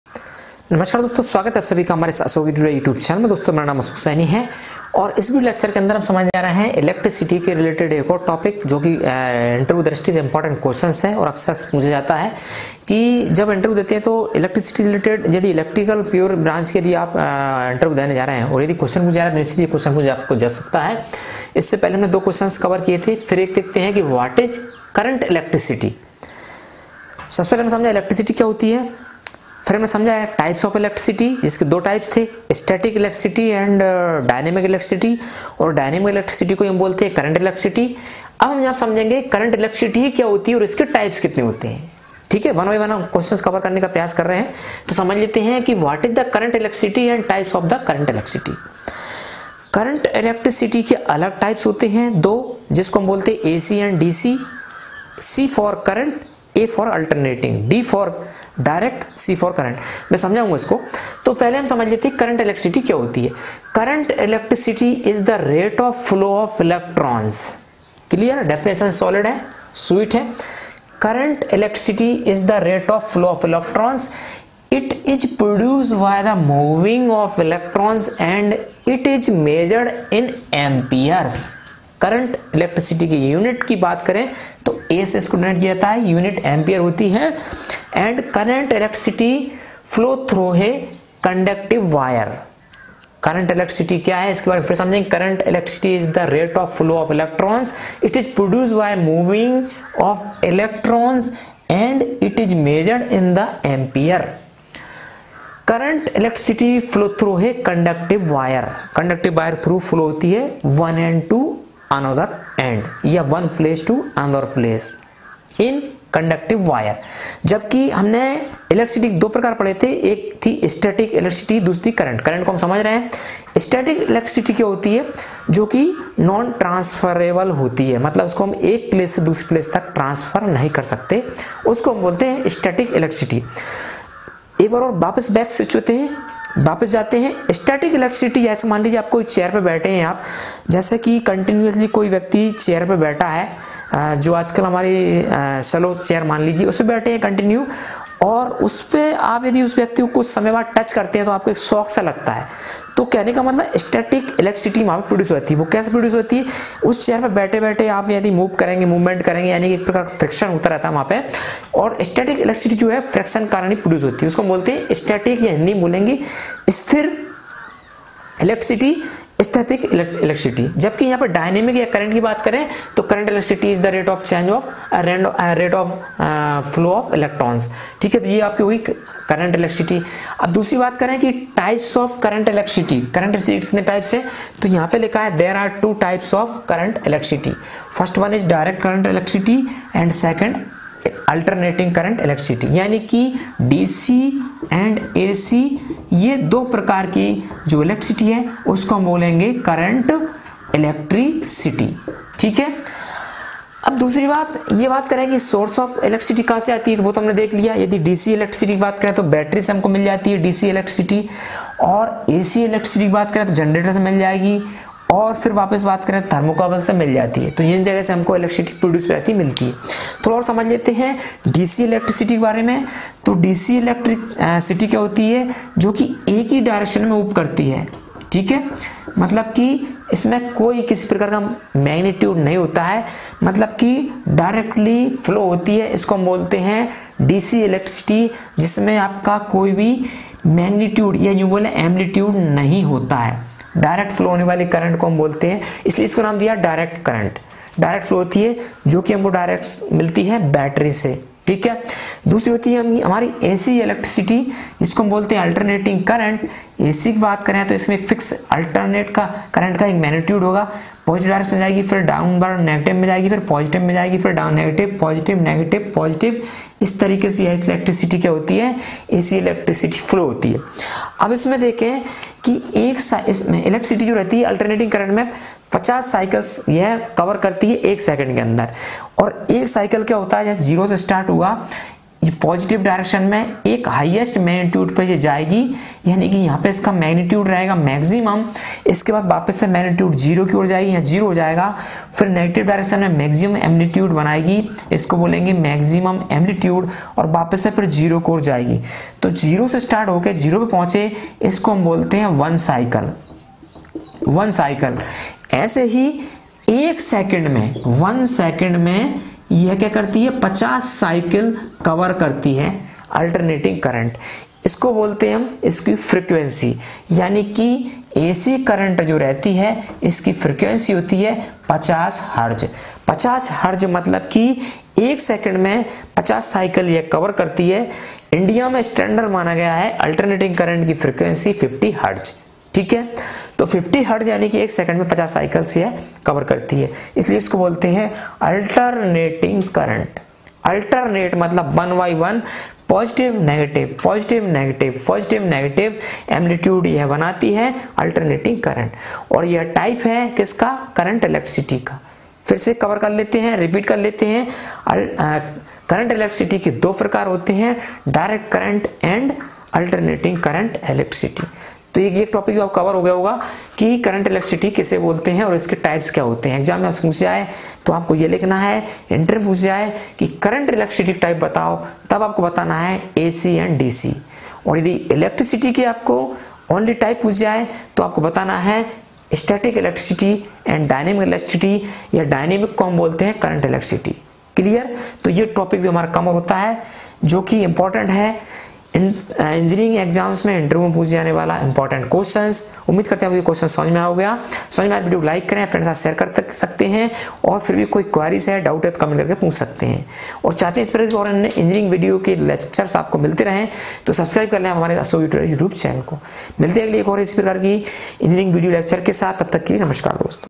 What is Current Electricity and types of Current Electricity in Hindi -
Topics Covered in This Video:
1. What is Current Electricity.
2. Types of Current Electricity.
3. What is AC Current Electricity.
4. What is DC Current Electricity.
5. Difference between AC and DC Current Electricity in Hindi.